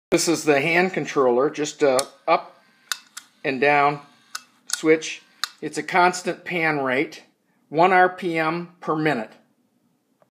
EBD 5000 w/hand controller
EBD 5000 panning unit with hand controller.  The panning unit is controlled with one toggle switch which remains centered.  Simply push up or down to pan and switch returns automatically to center position. The unit is powered by a nine volt battery which installs into a drawer mounted on the switch box.  Battery life is long because it only supplies power when unit is panning.